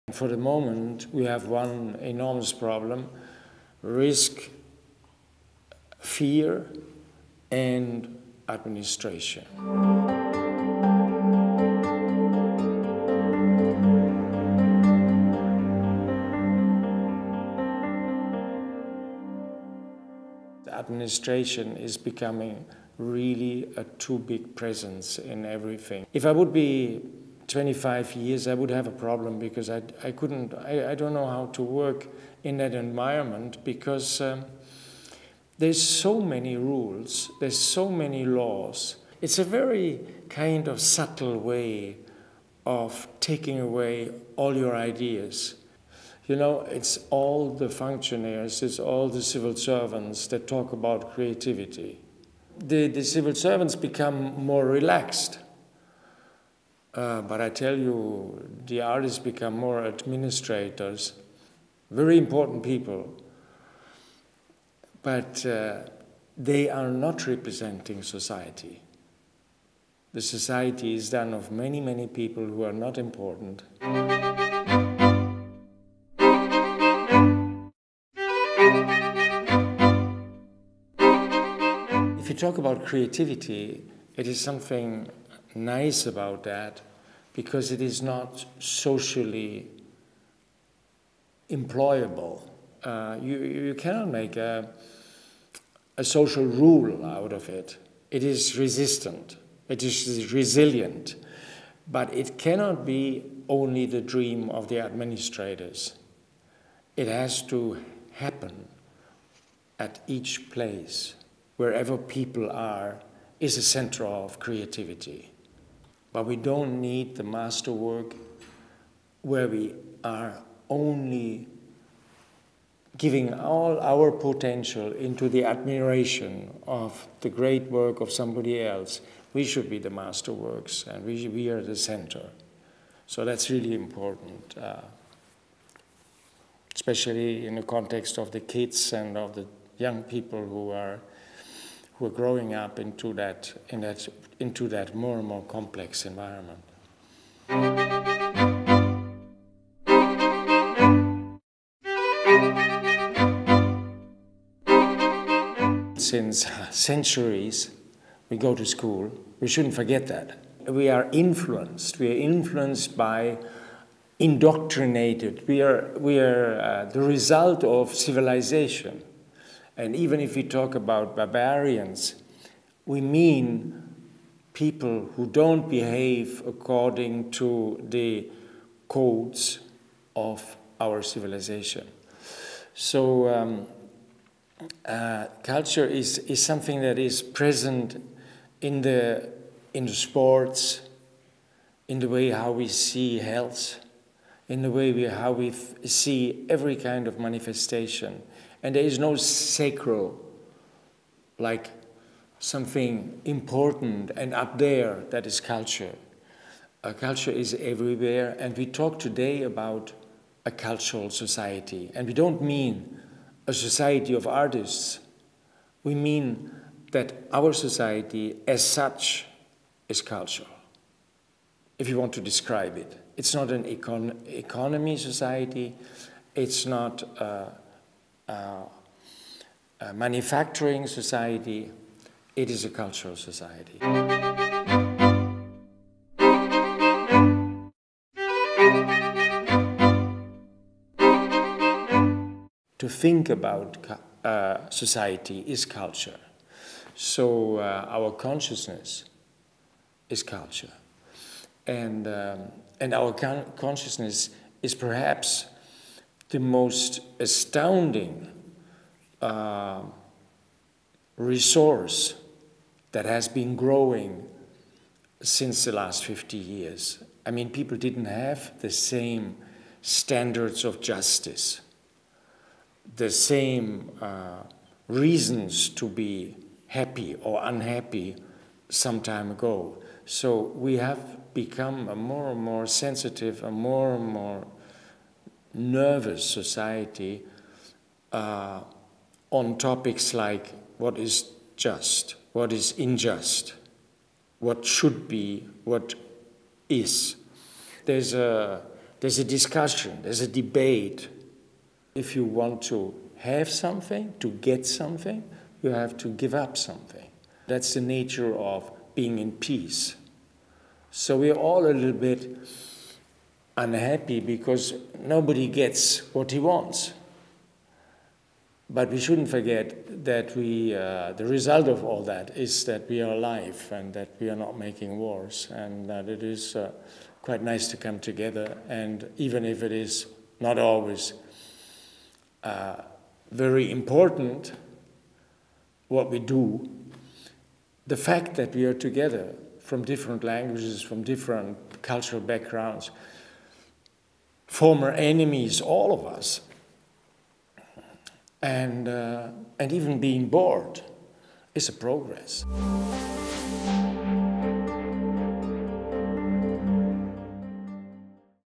Forum d´Avignon: Jochen Gerz: "Unser Bewusstsein ist Kultur"
Der Künstler und Professor Jochen Gerz führte auf dem Forum d`Avignon 2011 eine harte Bestandsaufnahme der Realität ins Felde: Kultur wird heute existentiell durch drei herrschende Faktoren bedroht: Risiko, Angst und Verwaltung.